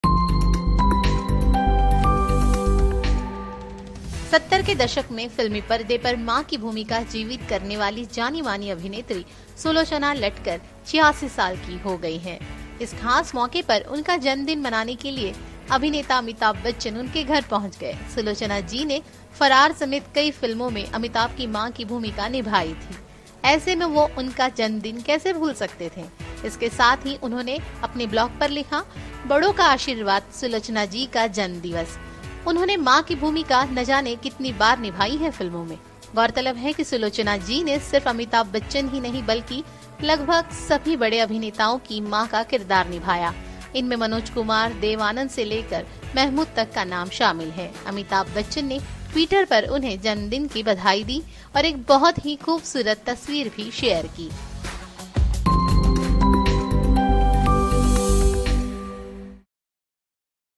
Amitabh Bachchan Meets Onscreen Mother on Her 86th Birthday
Megastar Amitabh Bachchan visited yesteryear actress Sulochana Latkar's home here to celebrate her 86th birthday. Sulochana also essayed the mothers's role of almost all the leading actors like Manoj Kumar, Dev Anand and Mehmood. The 72-year-old actor took to micro-blogging website Twitter and shared: "Sulochana ji happy 86th birthday! Played my Mother in endless films! Went to her home and greeted her! Sulochana ji, Mother to so many leading men in films - soft gentle and endearing... 86th birthday!".